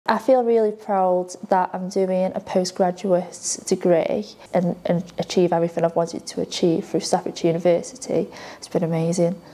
ProudToBeStaffs – Lauren (MA Social Work)
MA Social Work student, Lauren, talks about how proud she is to be a part of our Postgraduate Community at Staffordshire University and how she has managed to achieve everything she has wanted to whilst studying with us.